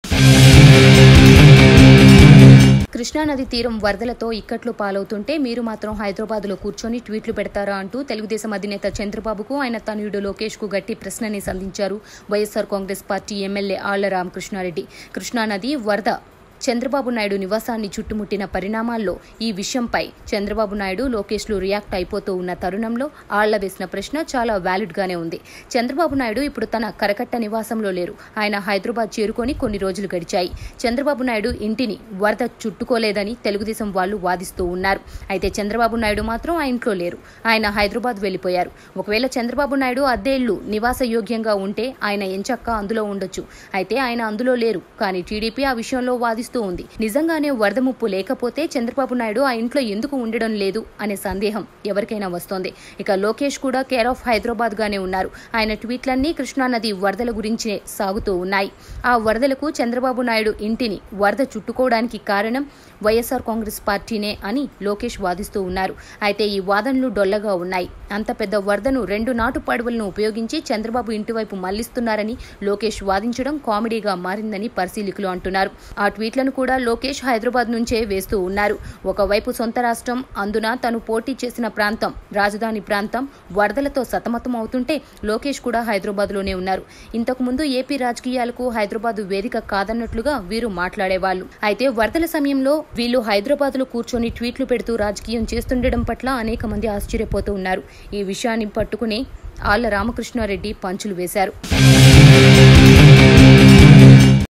వరద రాజకీయంపై టీడీపీకి వైసీపీ ఎమ్మెల్యే పవర్ పంచ్ ॥ YCP MLA Powerful Punch to TDP Leaders
వరద రాజకీయంపై టీడీపీకి వైసీపీ ఎమ్మెల్యే పవర్ పంచ్ ॥ YCP MLA Powerful Punch to TDP Leaders || Political Bench ||